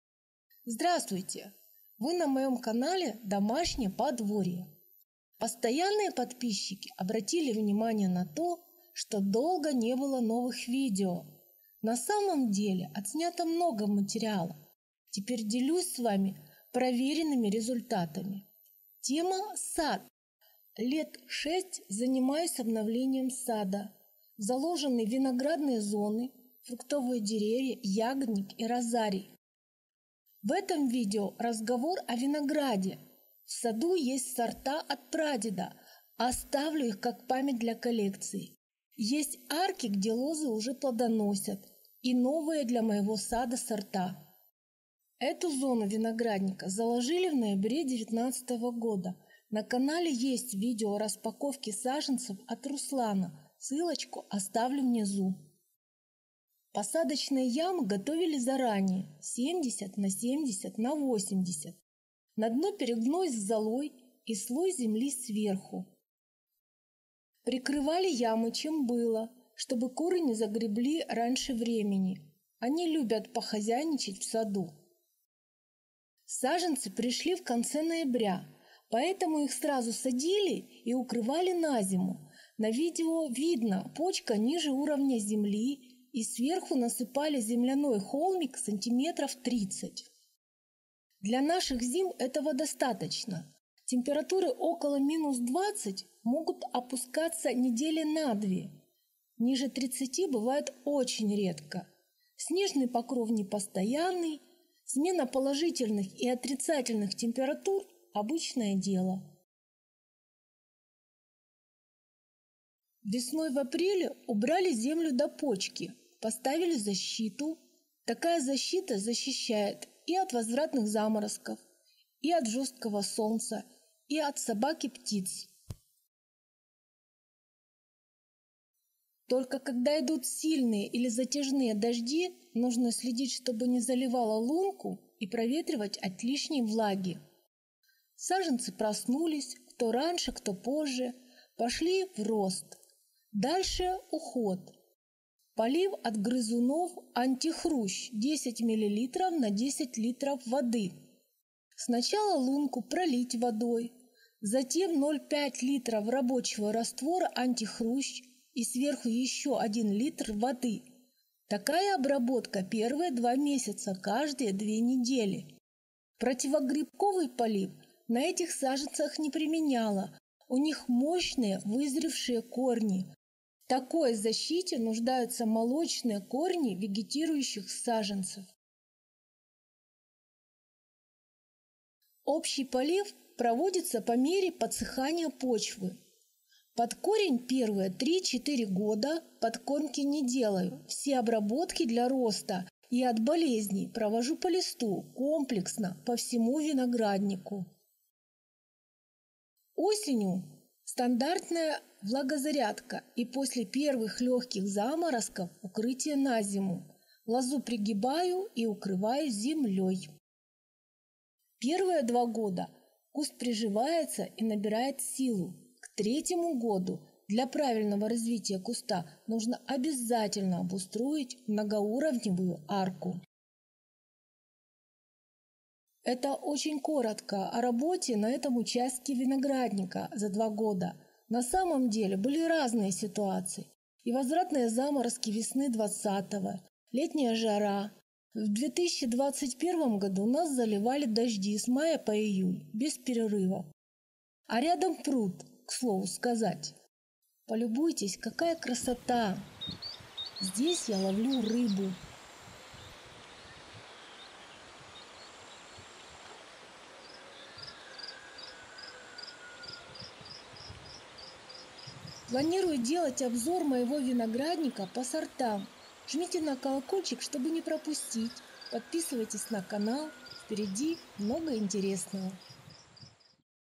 Осенняя посадка винограда.Закладка нового виноградника.
Закладка виноградника с нуля.